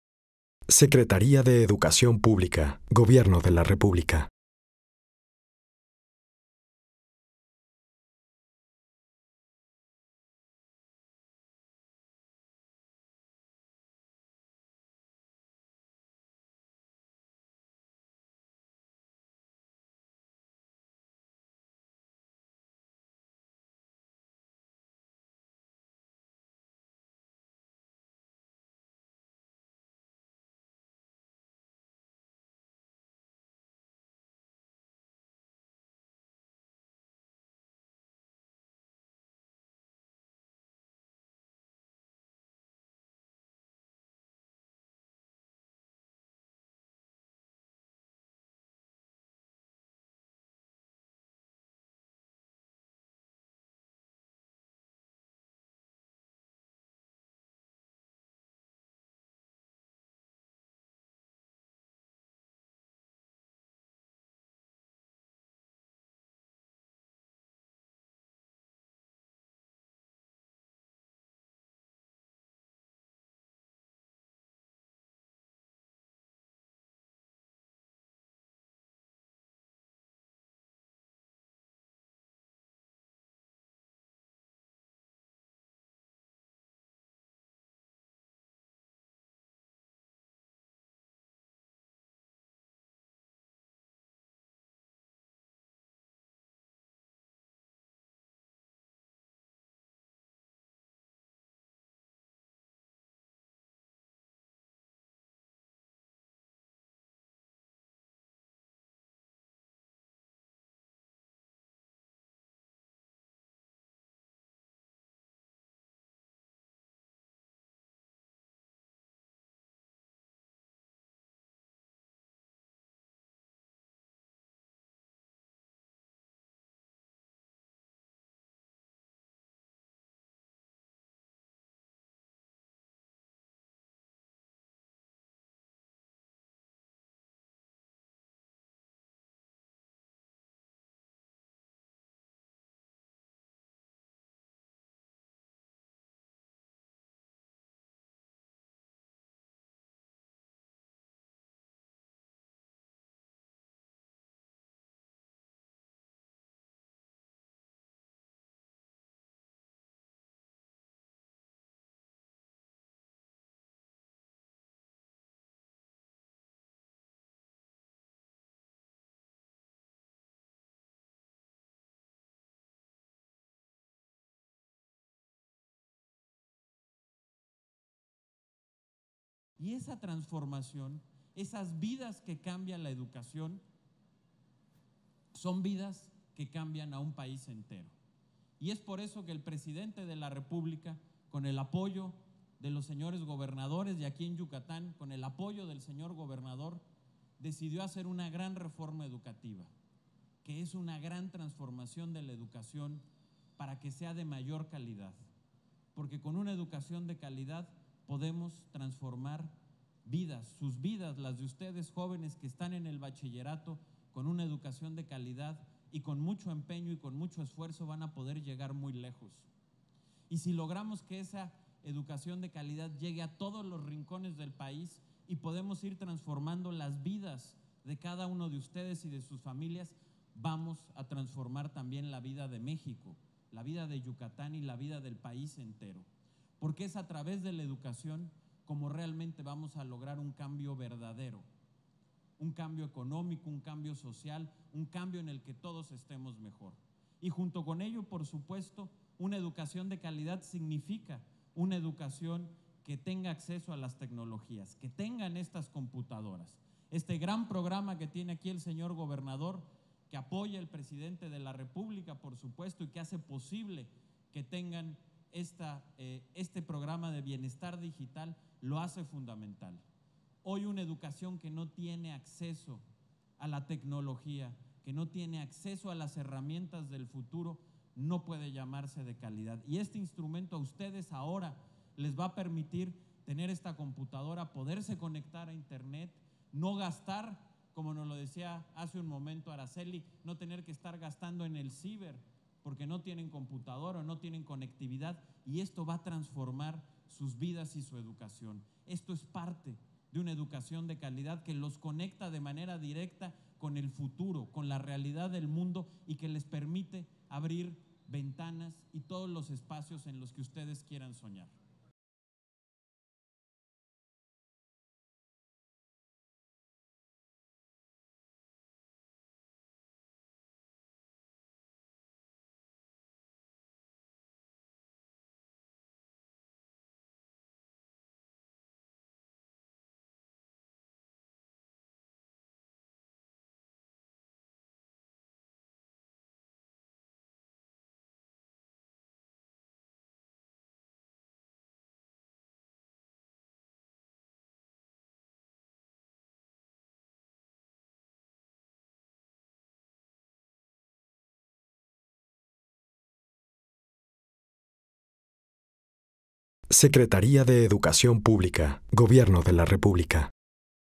Entrega de equipo de cómputo del programa Bienestar Digital
Mensaje del secretario de Educación Pública, Aurelio Nuño Mayer, durante la entrega de equipo de cómputo del programa Bienestar Digital, en el estado de Yucatán.